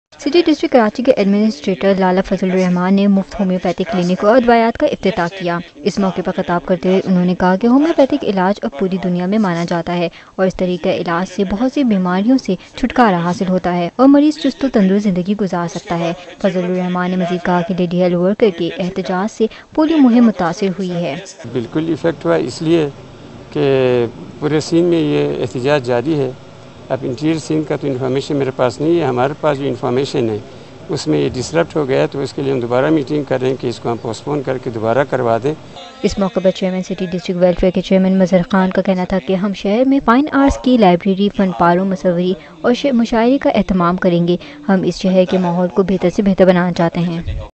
Khi CDGK Homeopathic#35C6D8.mp4
Lala Fazal ur Rehman opened free Homeopathic dispencary in Karachi